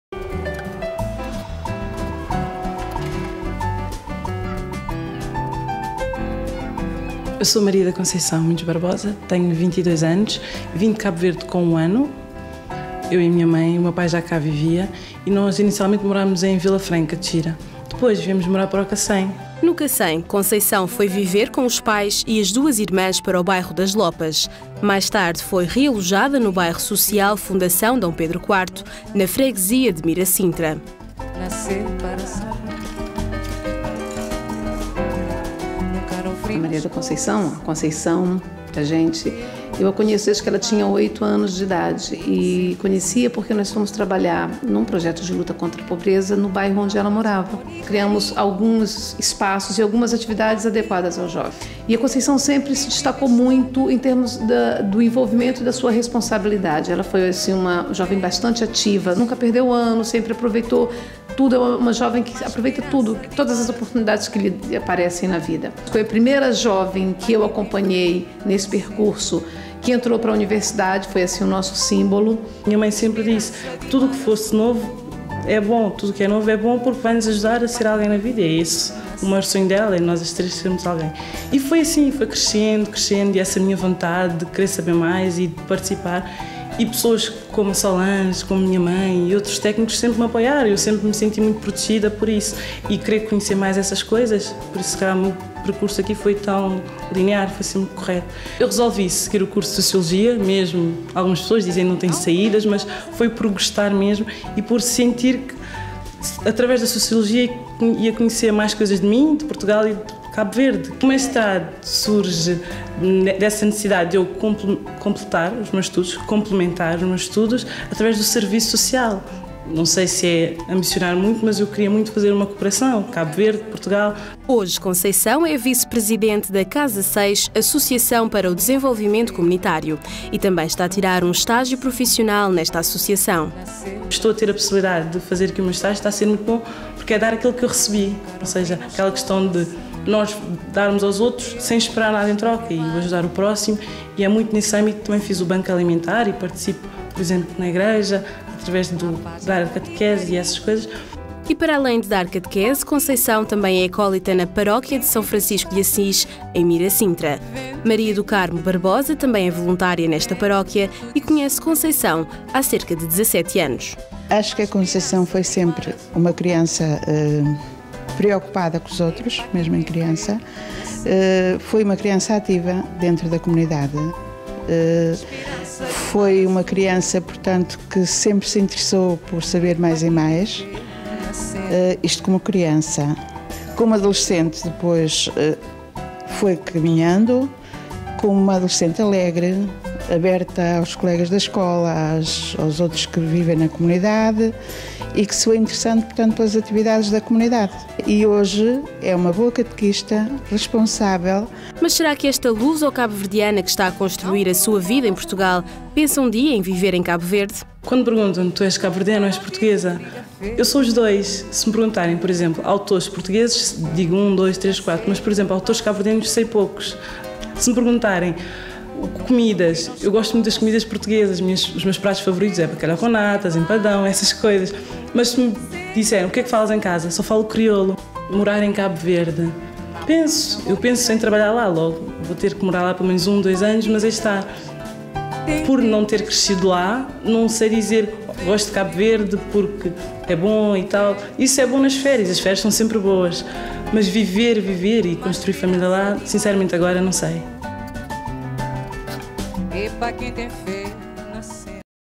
Maria da Conceição
Maria da Conceição veio de Cabo Verde com apenas 1 ano de idade, mas, culturalmente, considera-se tanto portuguesa, como cabo-verdiana. Em casa só fala crioulo. Fora de casa, a língua portuguesa lhe possibilitou tirar o curso de sociologia e a iniciar, agora, um mestrado em Serviço Social. Tem a família como um dos pilares de sustentação para a sua vida e sempre se envolveu em trabalhos de voluntariado. Espera, com isso, um dia poder regressar à Cabo-Verde para ajudar o seu país de origem.